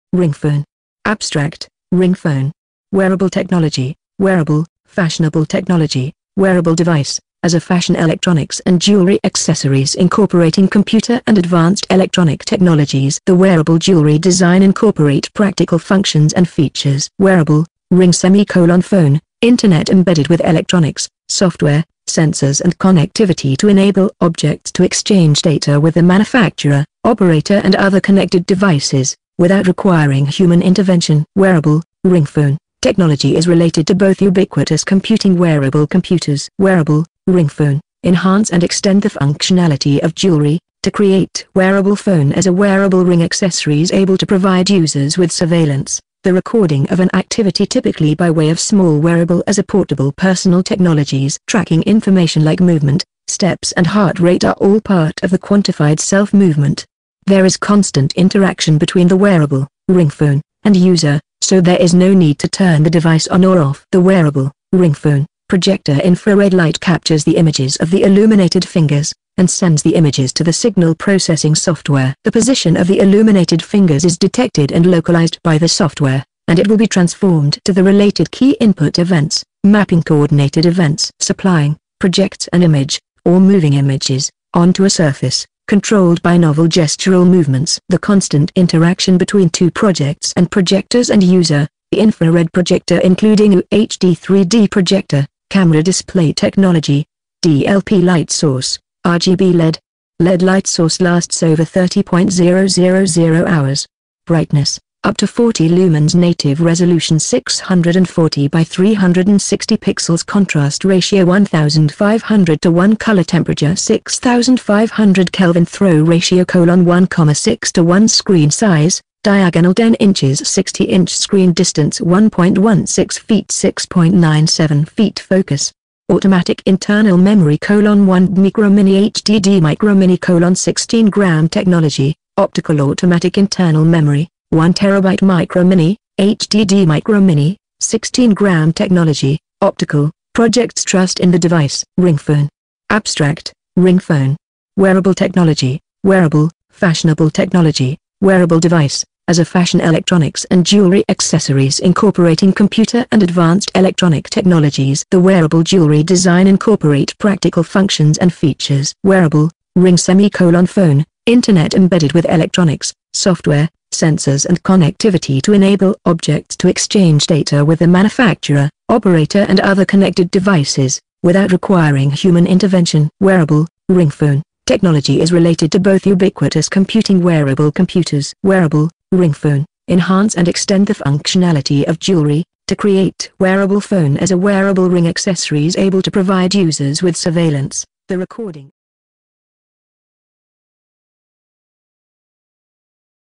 RINGPHONE
WEARABLE  PC &RING&PHONE+PROJECTOR IN ONE  MOBILE DEVICE WIRELESSLY
DESIGN BY JERMAINE MORTON